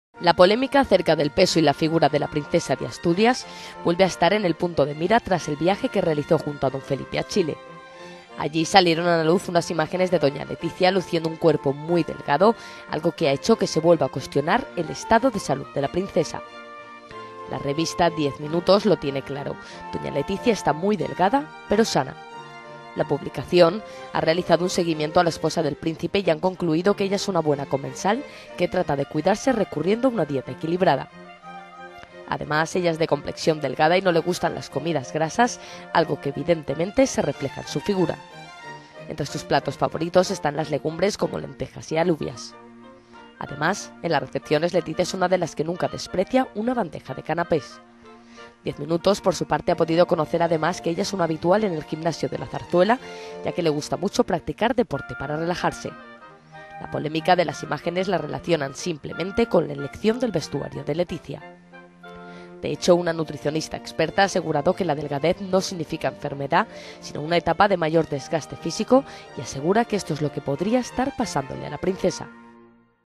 Letizia en el punto de mira por su delgadez
La polémica acerca del peso y la figura de la Princesa de Asturias vuelve a estar en el punto de mira tras el viaje que realizó junto a Don Felipe a Chile. Allí salieron a la luz unas imágenes de Doña Letizia luciendo un cuerpo muy delgado, algo que ha hecho que se vuelva a cuestionar el estado de salud de la Princesa.